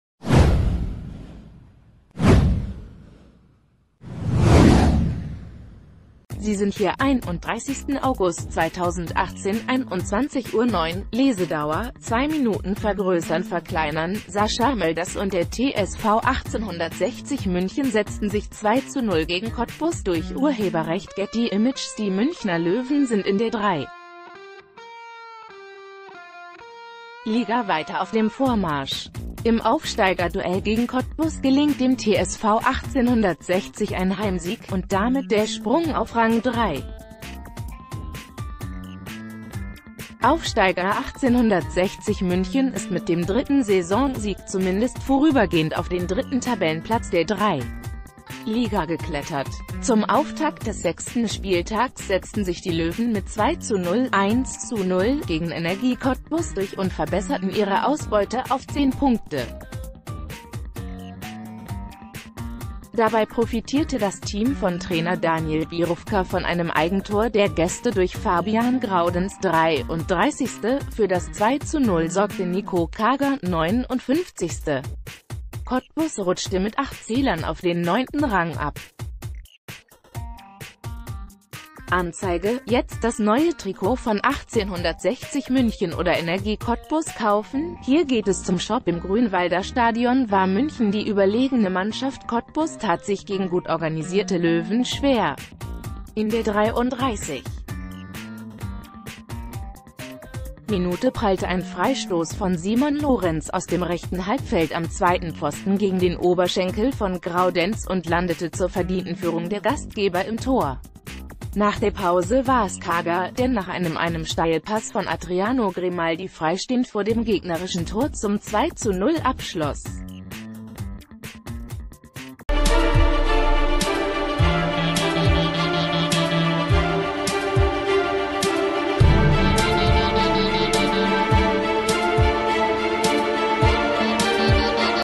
3. Liga: TSV 1860 besiegt Energie Cottbus 2:0
3. Liga: TSV 1860 besiegt Energie Cottbus 2:0
3. Liga: TSV 1860 besiegt Energie Cottbus 2:0

Sie sind hier: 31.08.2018 / 21:09 Uhr / Lesedauer: 2 Minuten vergrößernverkleinern Sascha Mölders und der TSV 1860 München setzten sich 2:0 gegen Cottbus durch © Getty Images Die Münchner Löwen sind in der 3. Liga weiter auf dem Vormarsch. Im Aufsteiger-Duell gegen Cottbus gelingt dem TSV 1860 ein Heimsieg - und damit der Sprung auf Rang 3. Aufsteiger 1860 München ist mit dem dritten Saisonsieg zumindest vorübergehend auf den dritten Tabellenplatz der 3. Liga geklettert. Zum Auftakt des sechsten...